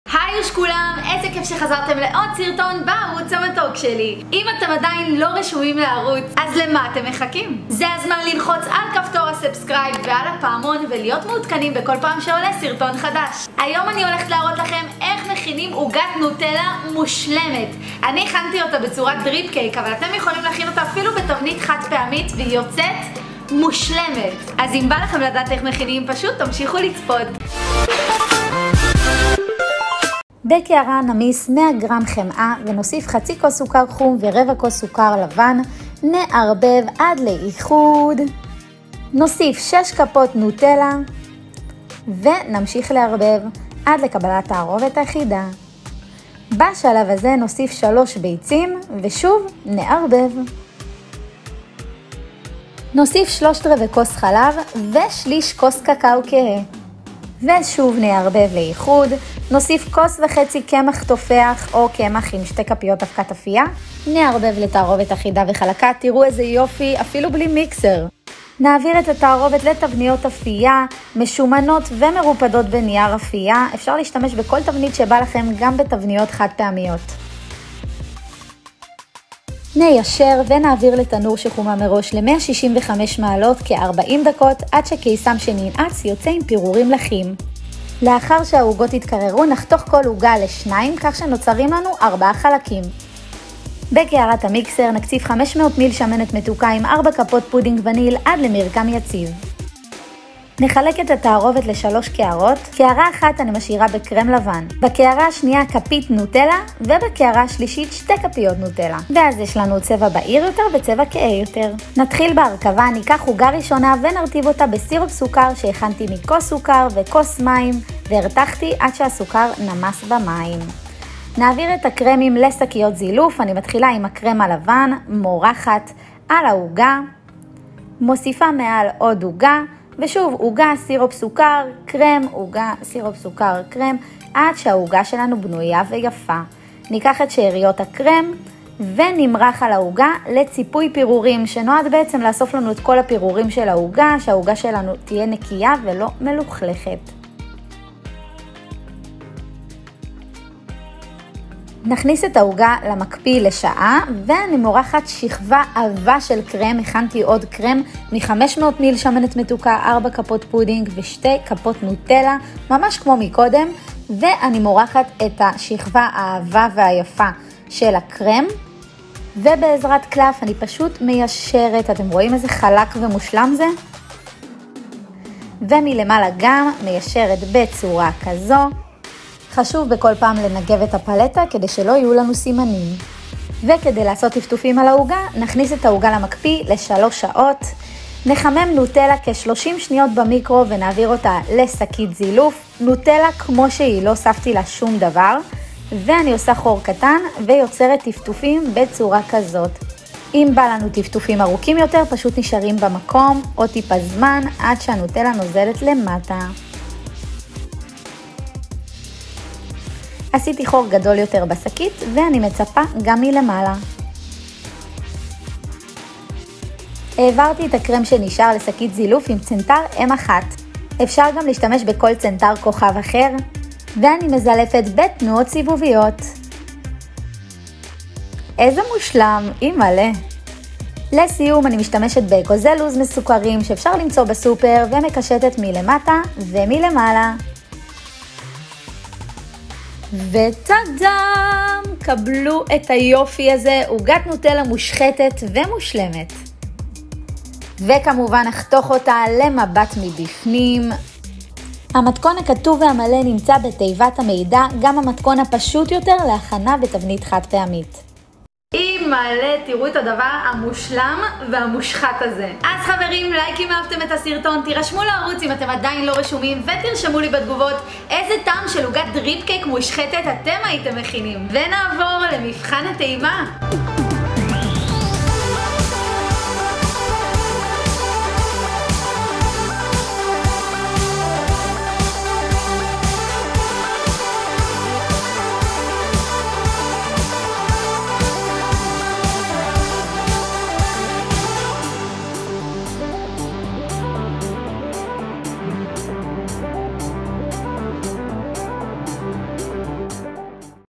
עוגת דריפקייק נוטלה מטורפת! | nutella cake
עוגת דריפקייק נוטלה הכי טעימה שתאכלו בחיים😉😉😉
אם לא בא לכם להשקיע בדריפקייק - אפשר לעשות את העוגה גם בתבנית רגילה או חד פעמית או 2 אינגליש קייק ולעשות שכבה אחת של קרם מעל (500 מל שמנת + 4 כפות פודינג+2 כפות נוטלה) ולקשקש מעל עם נוטלה:)

אם אהבתם את הסרטון ולא לשכוח להרשם לערוץ אם אתם עדיין לא רשומים😜

מתכון לעוגת דריפקייק בקוטר 18 גובה 8 או קוטר 20 גובה 6:
מצרכים לבחושה:
100 גרם חמאה מומסת
חצי כוס סוכר חום
רבע כוס סוכר לבן
3 ביצים
3/4 כוס חלב
6 כפות נוטלה
כוס וחצי קמח תופח (או קמח רגיל עם 2 כפיות אבקת אפייה)
שליש כוס קקאו

לסירופ סוכר:
כוס מים
כוס סוכר
מרתיחים ביחד עד שהסוכר נמס במים ומקררים טיפונת (שיהיה חמים)

קרם למילוי:
500 מל שמנת מתוקה + 4 כפות פודינג וניל
מחלקים ל3 קערות, קערה אחת משאירים לבן
קערה שנייה מוסיפים כפית נוטלה
קערה שלישית מוסיפים 2 כפיות נוטלה:)
כך ניצור גווני אומברה בתוך העוגה.

קרם לציפוי:
500 מל שמנת מתוקה
4 כפות פודינג וניל
2 כפות נוטלה

(סך הכל במתכון- נצנצת נוטלה גדולה)

אופן ההכנה:
נערבב את החמאה המומסת עם הסוכרים , נוסיף נוטלה, ביצים ונערבב. נוסיף חלב וקקאו ונערבב
לסיום נוסיף את הקמח ונערבב לאיחוד.
נעביר לתבניות אפייה משומנות (אפשר לשים מסביב נייר אפייה, זה יעזור בחילוץ)

נאפה בתנור שחומם ל165 מעלות בערך 40 דקות עד שקיסם שנעץ יוצא עם פירורים לחים.

נקרר את העוגות לטמפרטורת החדר (אפשר אפילו להכין יום לפני)
נחתוך כל עוגה ל2 (כלומר נוצר 4 חלקים)

נכין את הקרמים.
נניח עוגה אחת, נרטיב בסירופ, נשים קרם לבן ונניח מעל עוד עוגה וכך הלאה עד שמרכיבים את כל העוגה.
נצפה אותה בשאריות הקרם ונכניס למקפיא לשעה.

נכין עוד קרם (כדאי להכין אותו ממש לפני שמצפים)
נצפה את העוגה ציפוי עבה עם מלא קרם.

נכניס למקפיא לפחות ל3 שעות לפני שעושים דריפ עם הנוטלה.

מחממים את הנוטלה במיקרו ומעבירים לשקית זילוף
מזלפים טפטופים:)

את שאריות הקרם מזלפים מעל❤️
שומרים במקרר עד 5 ימים... אפשר לשמור במקפיא עד שלושה שבועות ולהוציא למקרר 4 שעות לפני ההגשה.
תהנו מלאאאא ואל תשכחו שאם אתם עצלנים אפשר להכין אותה כמו עוגת שוקולד רגילה🤪🤪🤪🤪
אבל בתכלס אין לכם מה לעשות בבידוד אז תשקיעו😂💃🏻💃🏻💃🏻💃🏻

אוהבת מלאא❤️